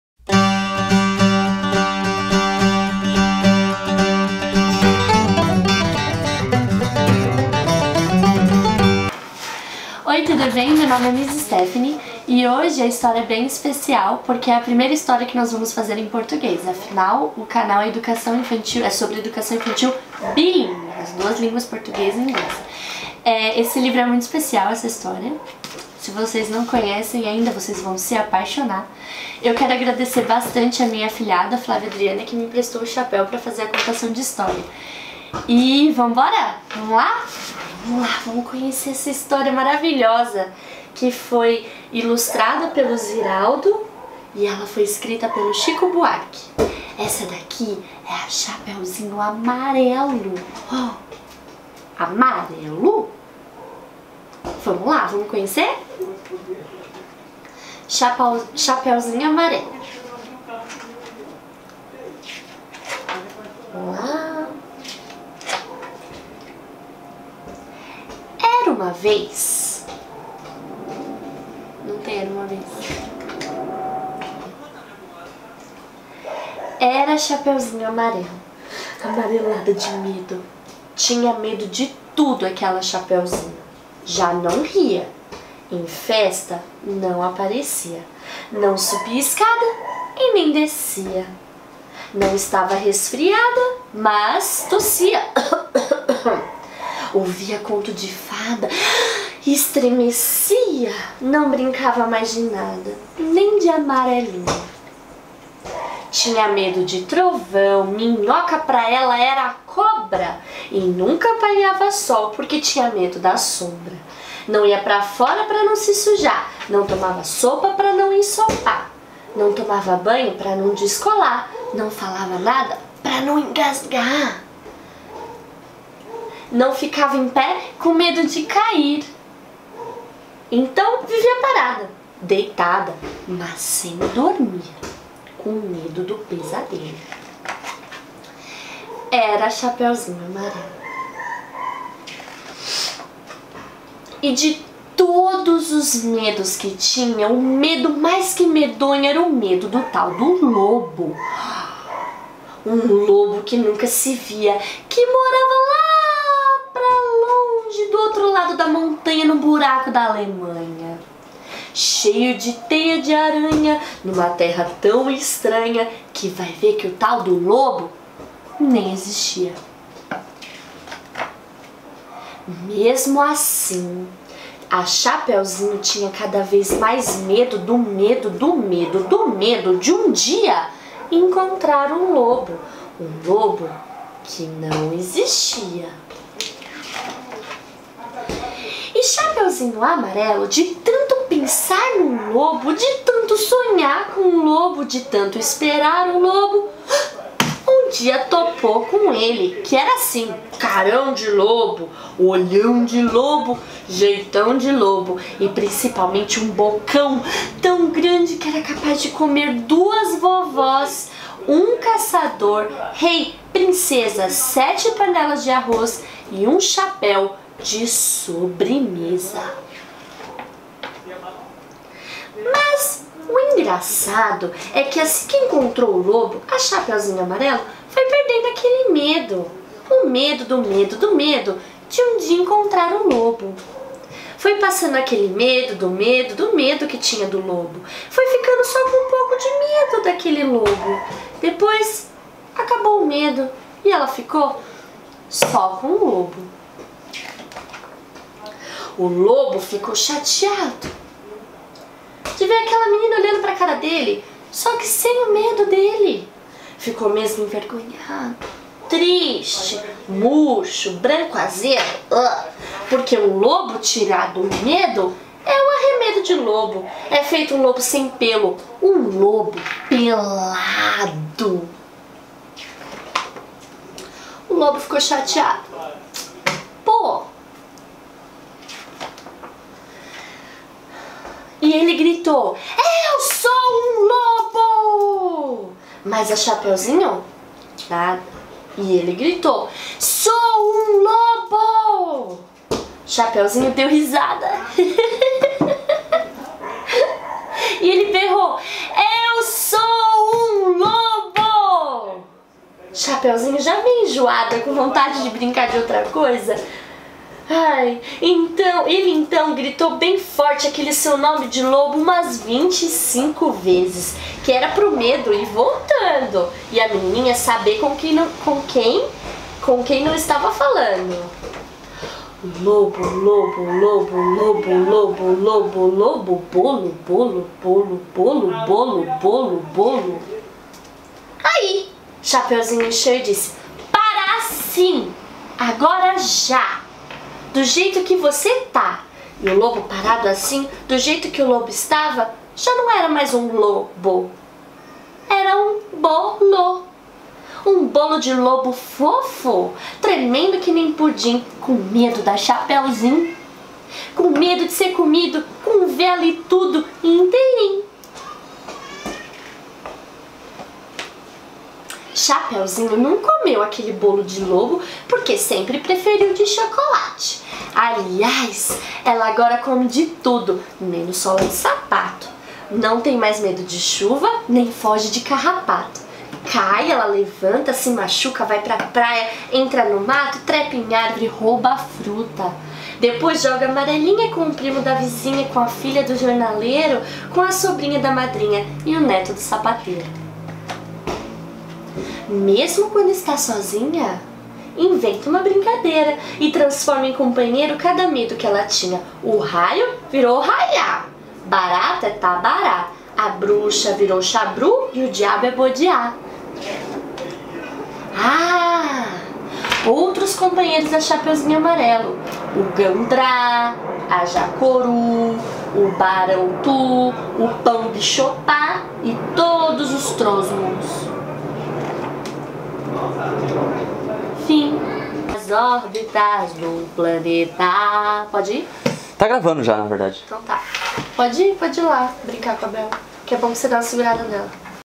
HORA DA HISTÓRIA: CHAPEUZINHO AMARELHO
Era uma vez uma menina que tinha medo de tudo!
Era amarelada de medo...

Um livro mais que fantástico: CHAPEUZINHO AMARELO de Chico Buarque, ilustrado por Ziraldo.

Perfeito para conversar sobre medos, como vencer o medo.
Uma conversa bem interessante e necessária de ter com as crianças.

Quais são os medos que seus filhos ou crianças tem?

Meu Instagram mudou! Agora é